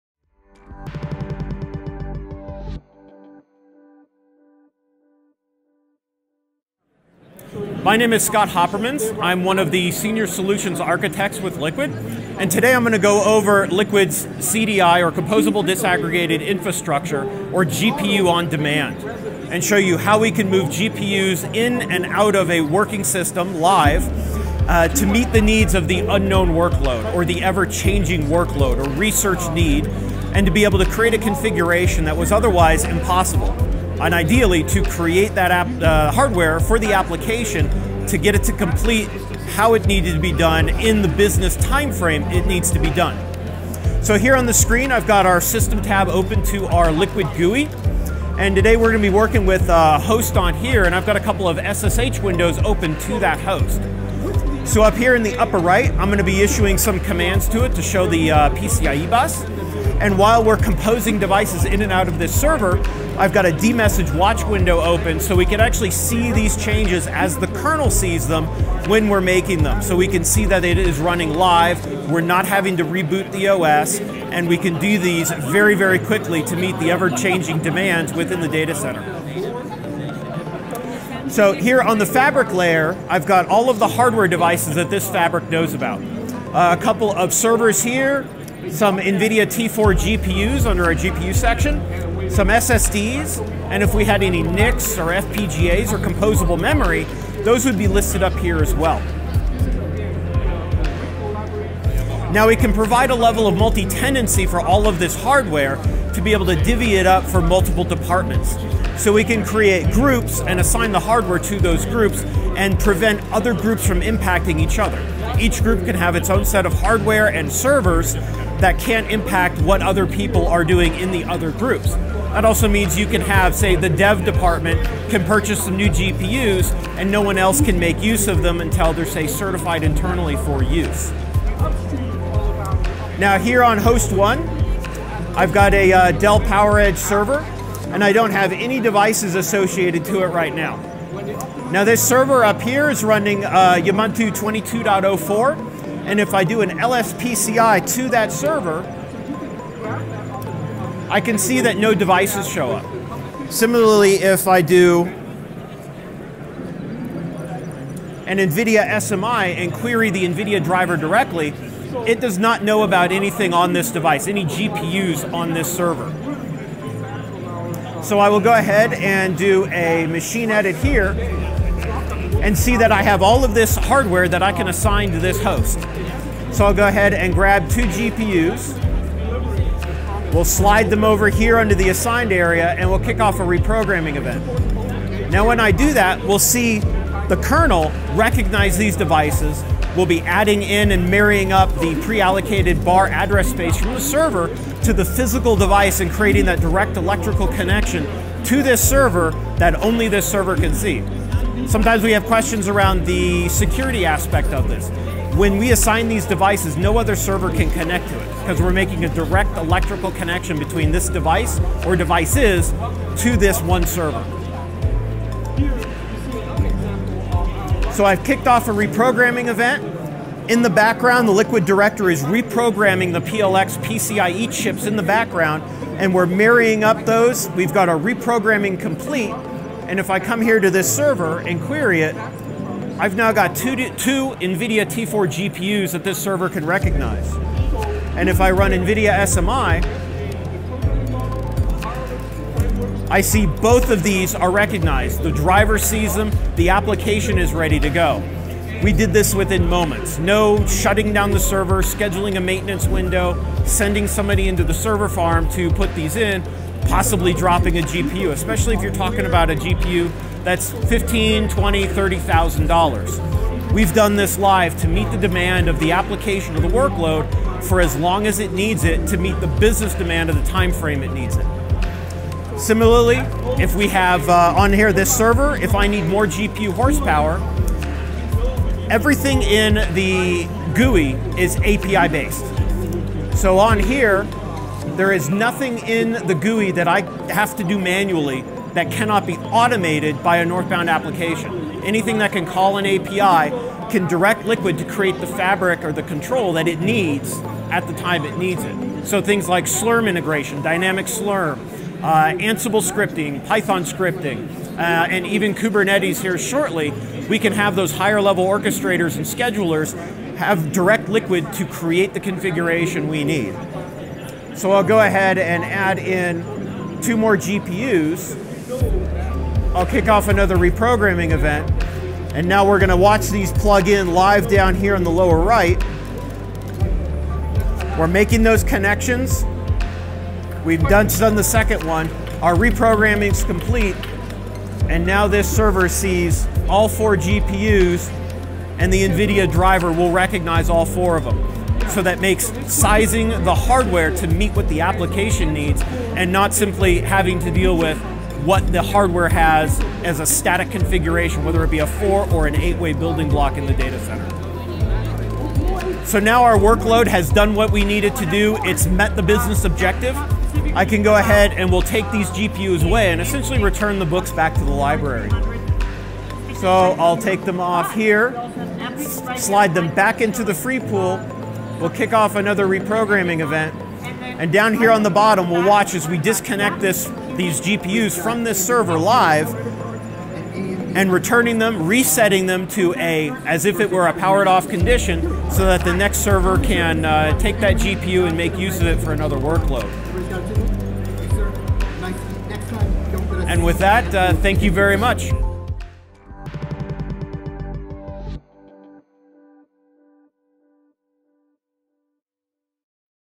SC23 Maximize GPU Density with Liqid Demo
Scott Houppermans, Senior Solutions Architect at Liqid, Demonstrates at SC23 how Liqid software boosts performance by allocating GPUs to a single server.